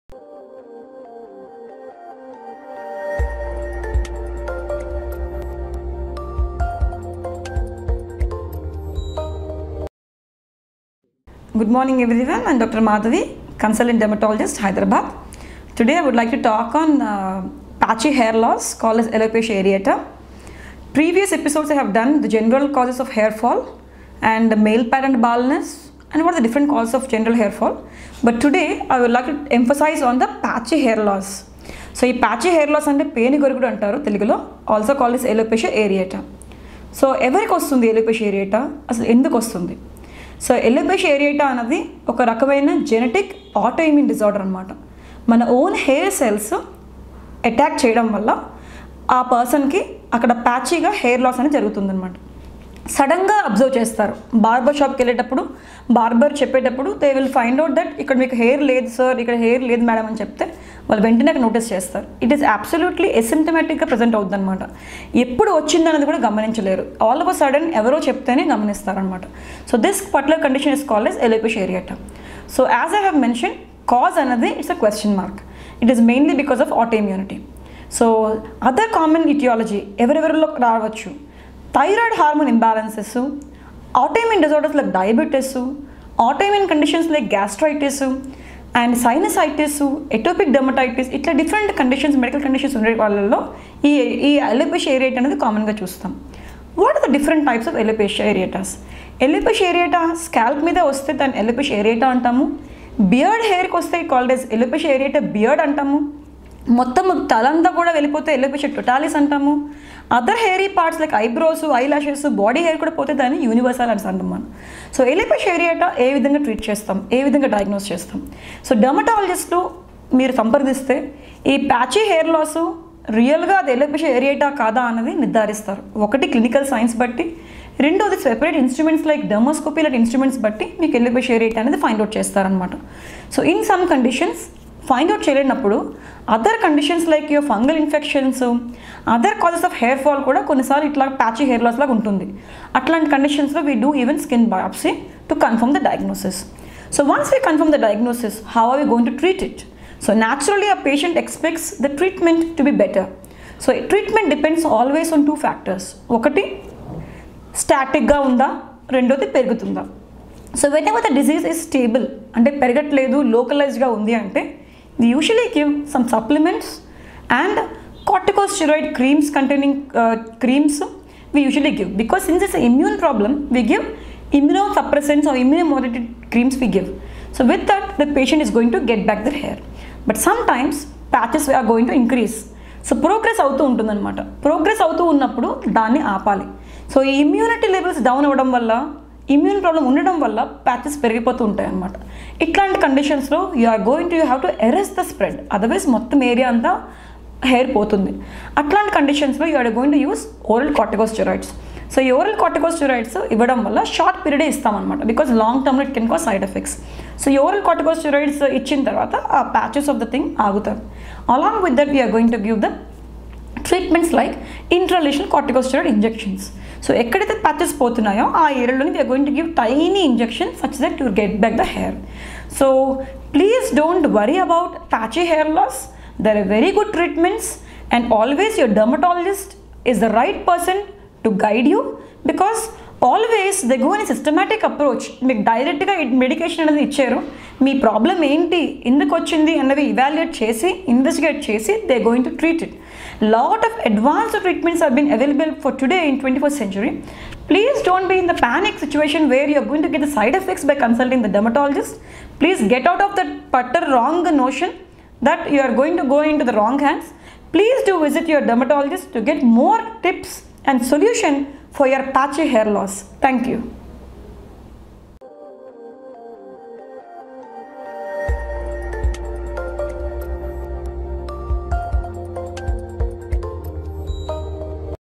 Patchy Hair Loss and its treatment by Dr. Madhavi pudi.
What is patchy hair loss, causes of it, and treatment by Dr. Madhavi Pudi.